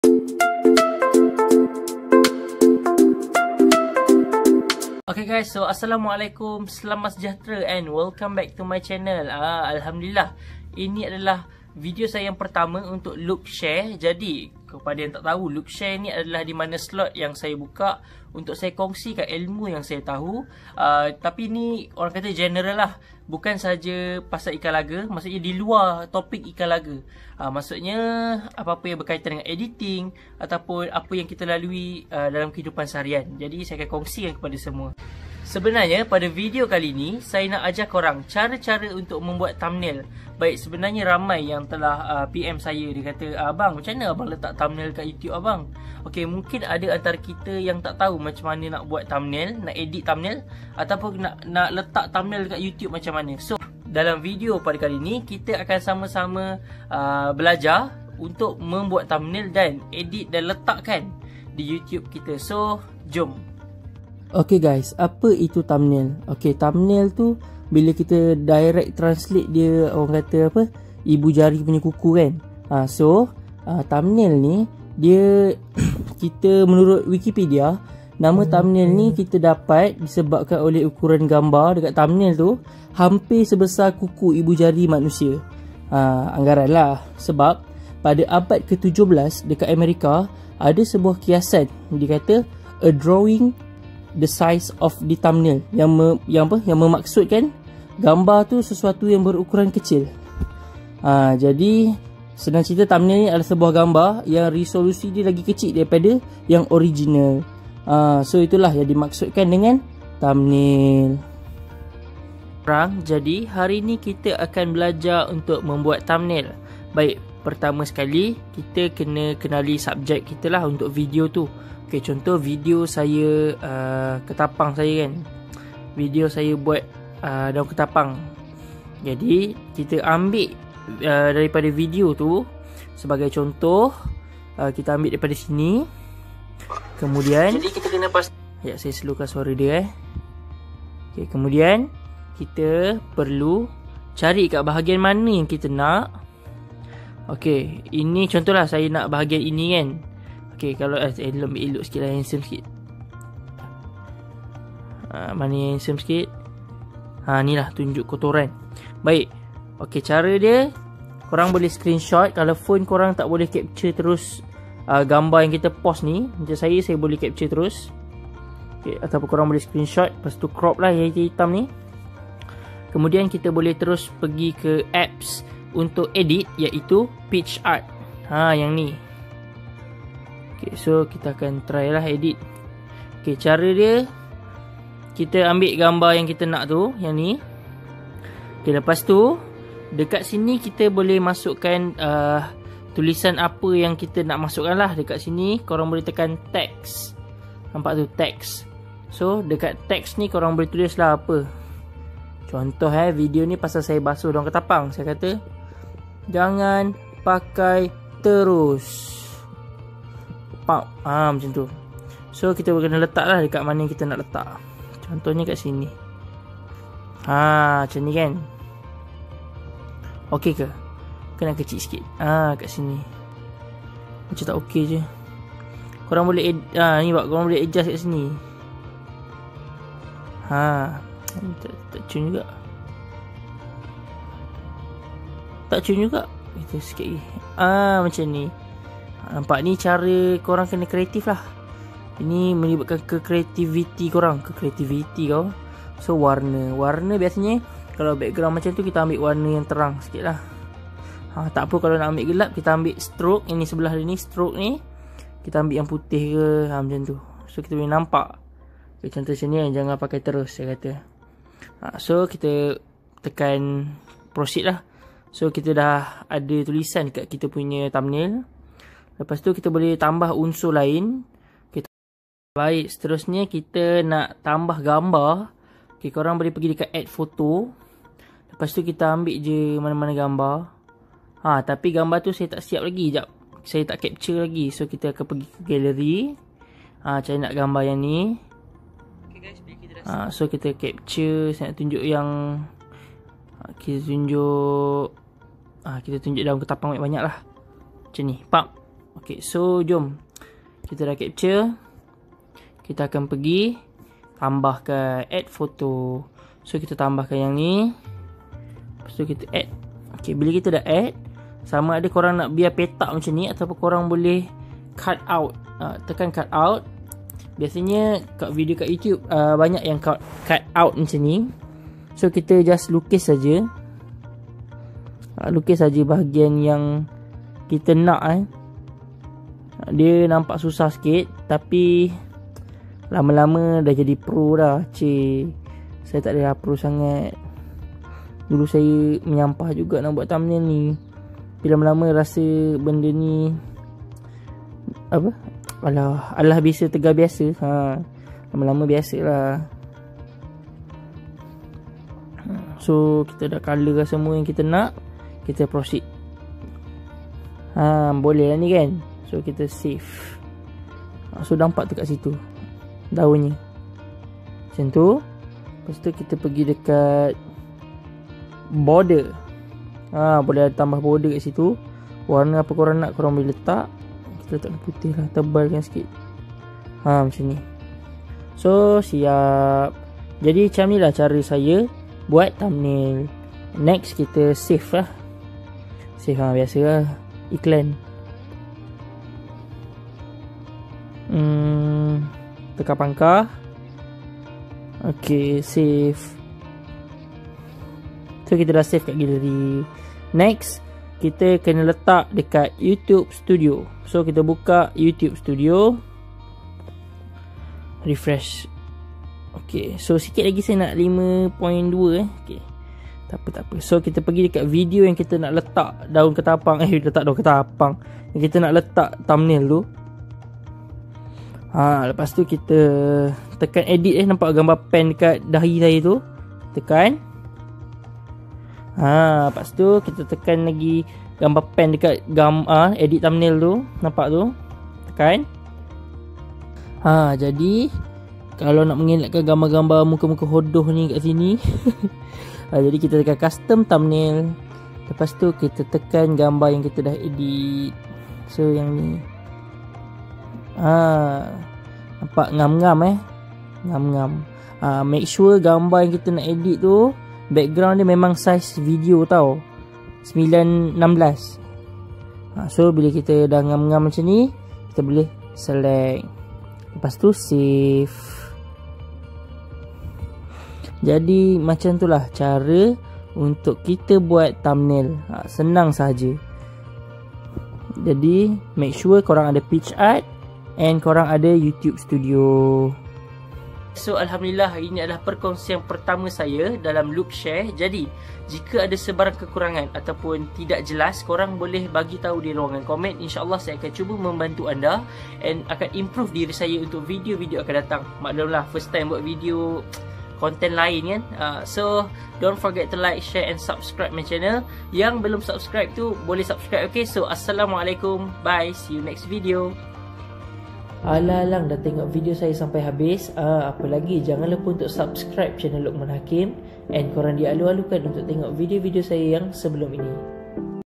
CARA MUDAH EDIT DAN SET THUMBNAIL MENGGUNAKAN TELEFON | Luq Share 01
Video kalini, saya nak share kan cara mudah yang saya selalu guna untuk mengedit thumbnail untuk video video saya. Hanya menggunakan Picsart untuk edit. Thumbnail yang menarik sangat penting kerana dapat menarik minat pada penonton.  Harap video ini dapat membantu pada yang baru nak menjinak diri dalam dunia youtube. Selamat Mencuba...

Apps yang digunakan - Picsart